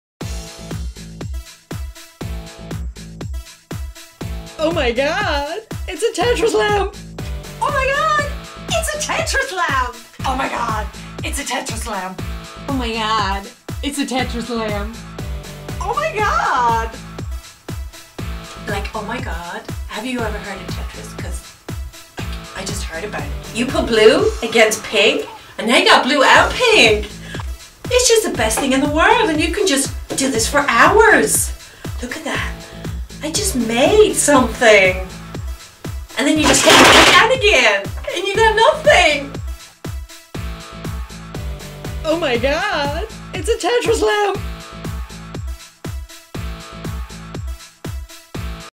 OMG Tetris lamp
Rita Ann (Outrageous Productions) talks tetris lamp